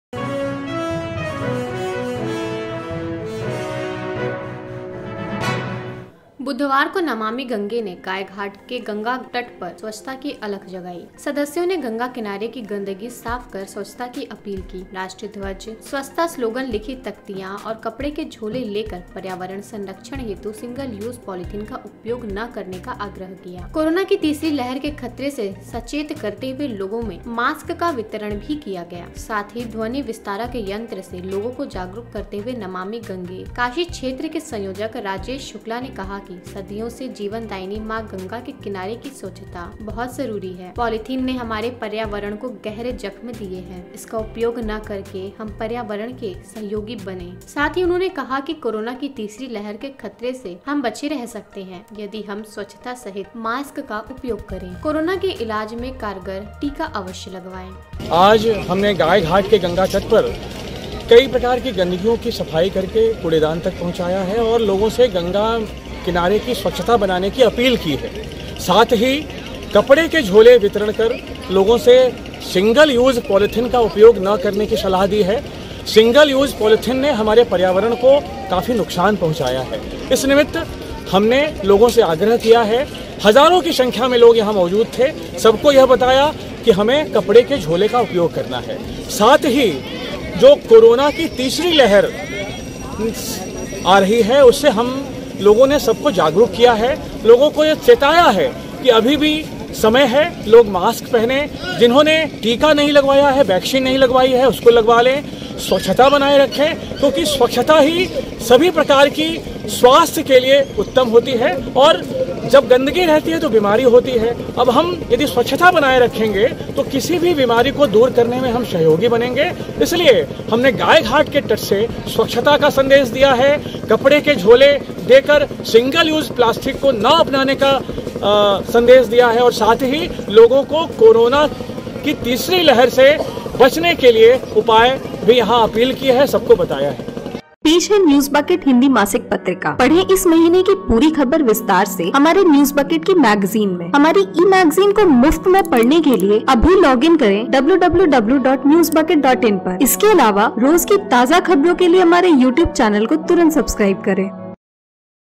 नमामि गंगे ने Ganga तट पर की स्वच्छता की शुरुवात, Corona की 3rd Wave को लेकर भी किया जागरूक
बुधवार को नमामि गंगे ने गाय घाट के गंगा तट पर स्वच्छता की अलख जगाई। सदस्यों ने गंगा किनारे की गंदगी साफ कर स्वच्छता की अपील की। राष्ट्रध्वज, स्वच्छता स्लोगन लिखी तख्तियां और कपड़े के झोले लेकर पर्यावरण संरक्षण हेतु सिंगल यूज़ पॉलीथिन का उपयोग न करने का आग्रह किया। कोरोना की तीसरी लहर के खतरे से सचेत करते हुए लोगों में मास्क का वितरण किया गया। ध्वनि विस्तारक यंत्र से लोगों को जागरूक करते हुए नमामि गंगे काशी क्षेत्र के संयोजक राजेश शुक्ला ने कहा कि सदियों से जीवनदायिनी मां गंगा के किनारे की स्वच्छता बहुत जरूरी है। Watch the complete Video For More Details!

Gadgets We Use:

Green Chroma